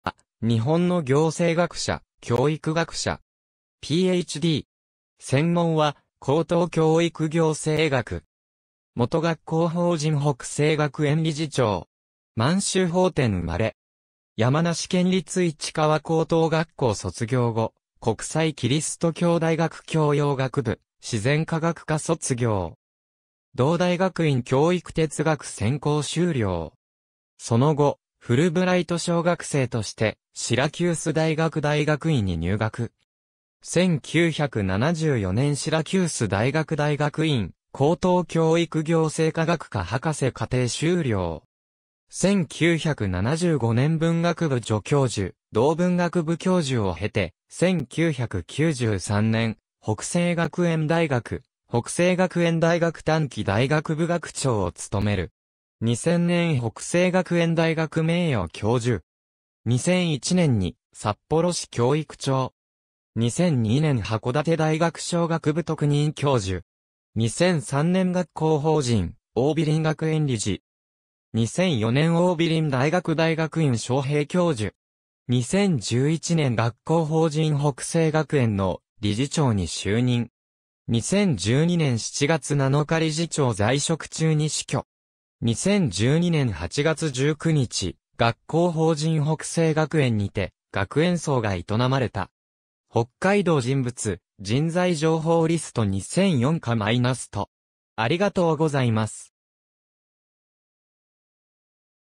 土橋信男
土橋信男（どばし のぶお、1936年-2012年）は、日本の行政学者・教育学者。
Ph.D.（シラキュース大学）。
専門は、高等教育行政学。
元学校法人北星学園理事長。
満州奉天生まれ。
山梨県立市川高等学校卒業後、国際基督教大学教養学部自然科学科卒業。
同大学院教育哲学専攻修了。
その後、フルブライト奨学生としてシラキュース大学大学院に入学。
1974年シラキュース大学大学院高等教育行政科学科博士課程修了。
1975年文学部助教授、同文学部教授を経て、1993年北星学園大学・北星学園大学短期大学部学長を務める（～2000年）。
2000年北星学園大学名誉教授。
2001年に札幌市教育長（～2002年）。
2002年函館大学商学部特任教授（～2011年）。
2003年学校法人桜美林学園理事（～2011年）。
2004年桜美林大学大学院招聘教授（～2011年）。
2011年学校法人北星学園の理事長に就任。
2012年7月７日理事長在職中に死去。
2012年8月19日学校法人北星学園にて学園葬が営まれた。
『北海道人物・人材情報リスト2004　か－と』（日外アソシエーツ編集・発行、2003年）